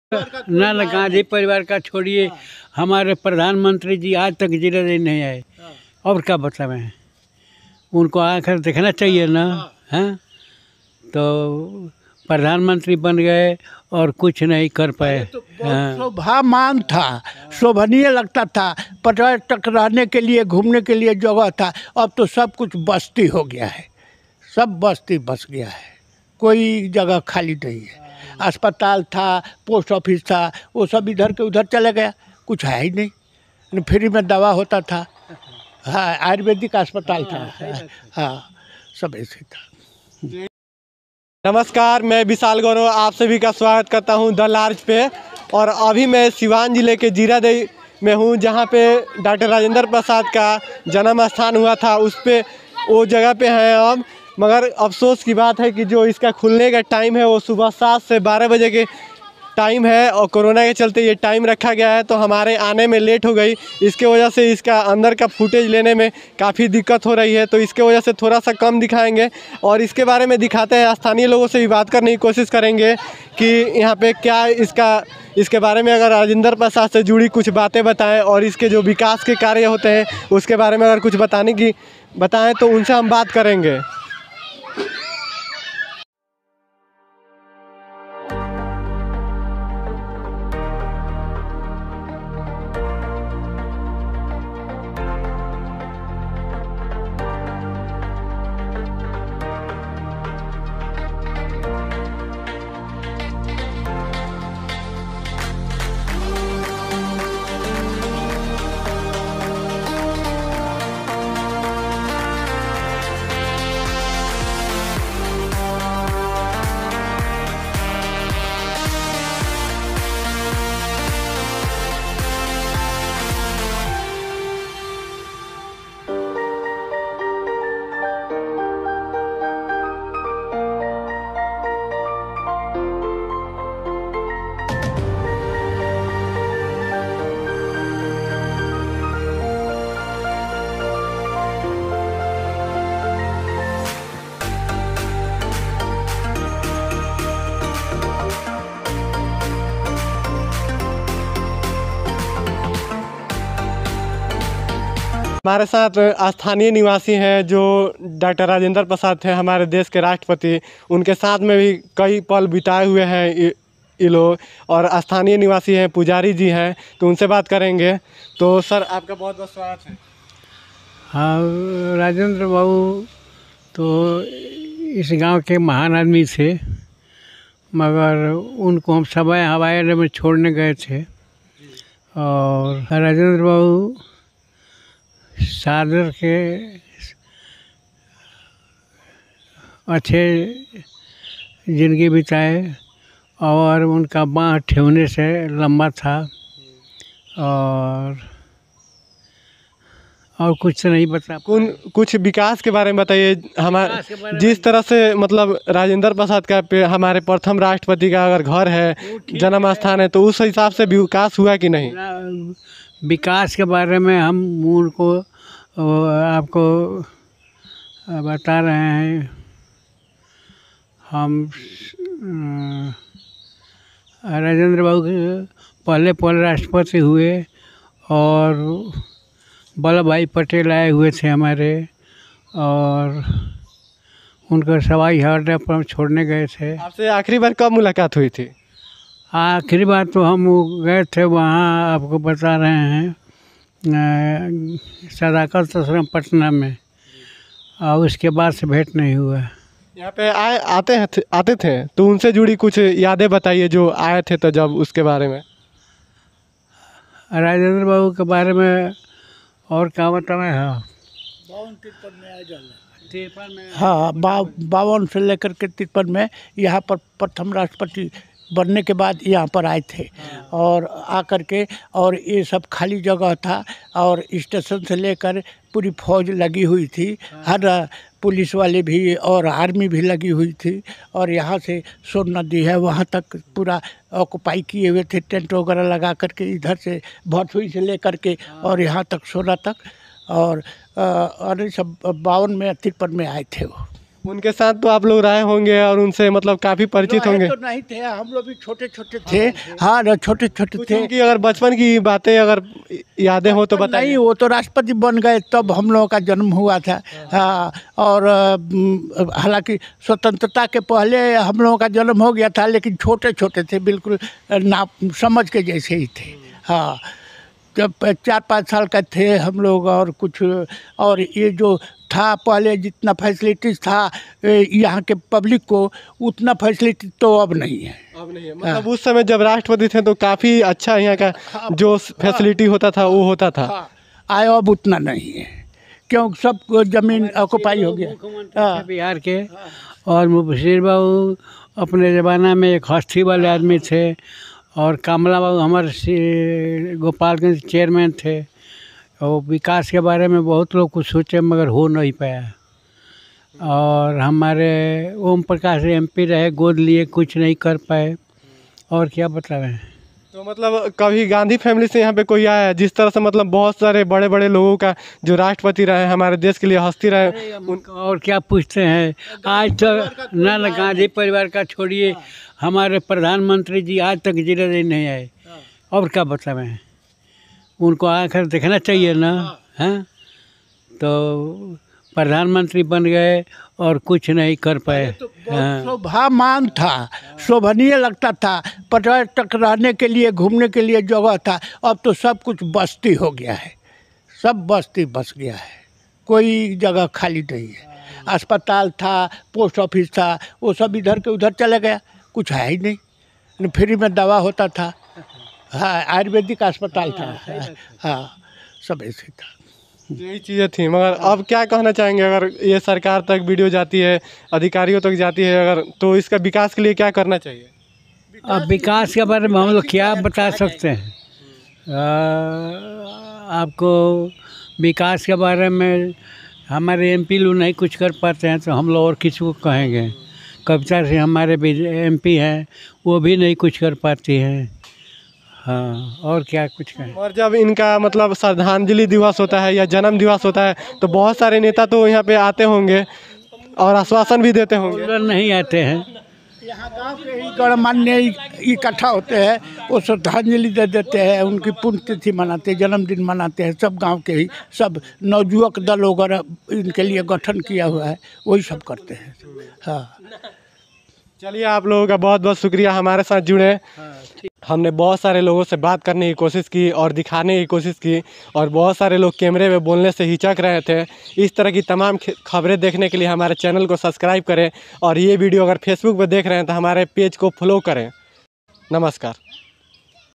प्रधानमंत्री आज तक नहीं आए जिरादेई ? नेताओं ने गोद लिया कुछ नहीं हुआ !
Edit:- Vishal Gaurav